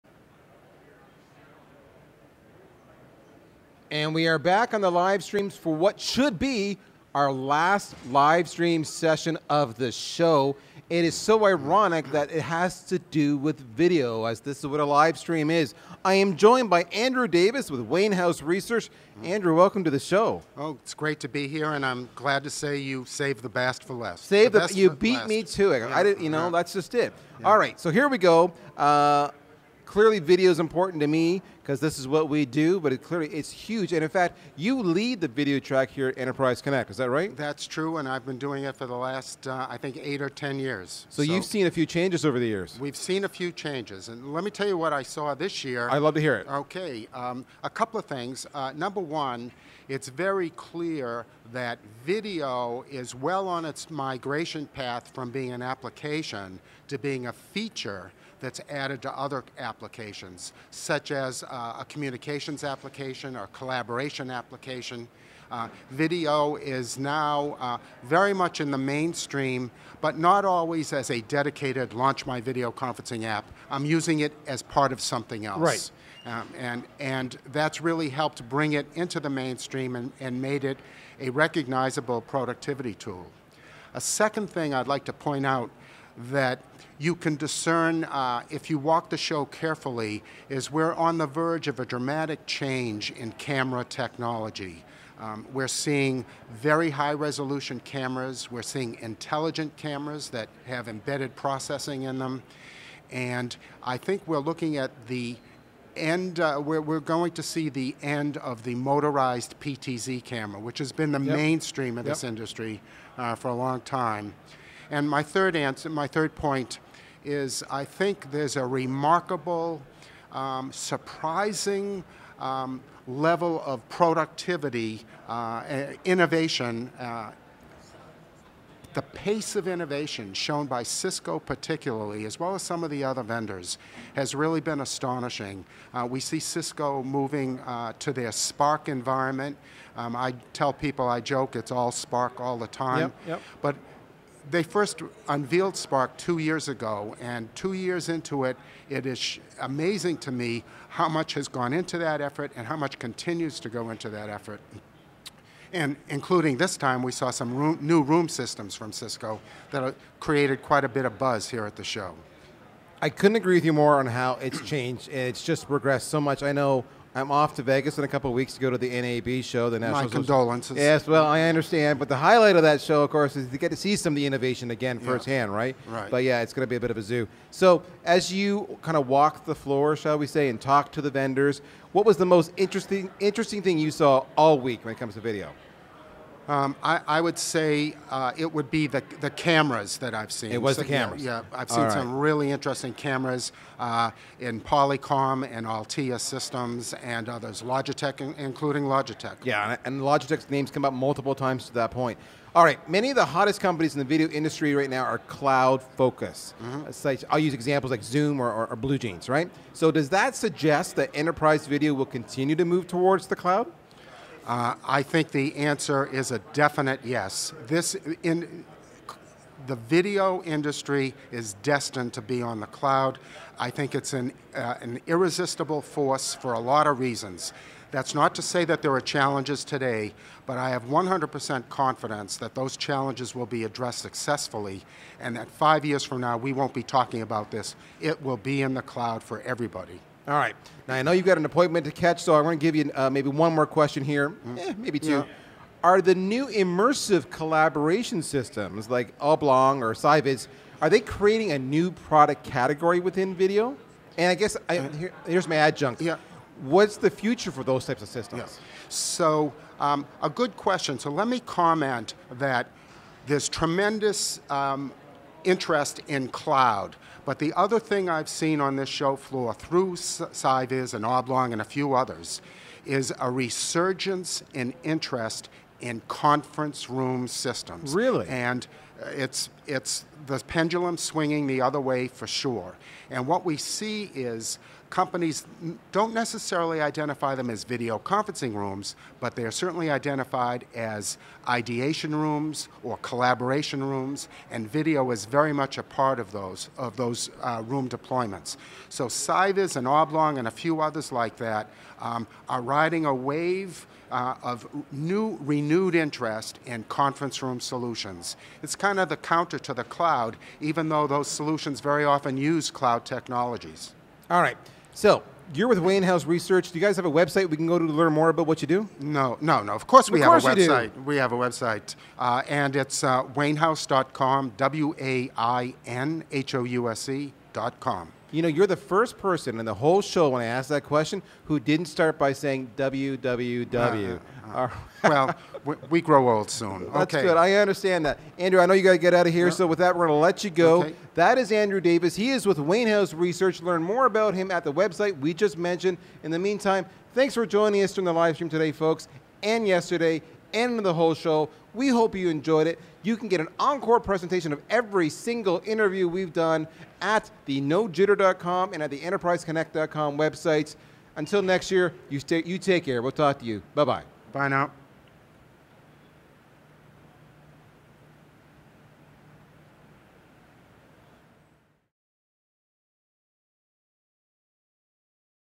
Enterprise Connect 2017 Live Interview Andrew Davis Wainhouse Research
Interview with Andrew Davis, Partner, Wainhouse Research

Topics Discussed:
Changes in Video in the last year

Most Interesting Video findings at Enterprise Connect 2017

Will Enterprise Video continue to move to the cloud?

Are immersive collaboration systems creating a new product category within video?

The future of immersive collaboration systems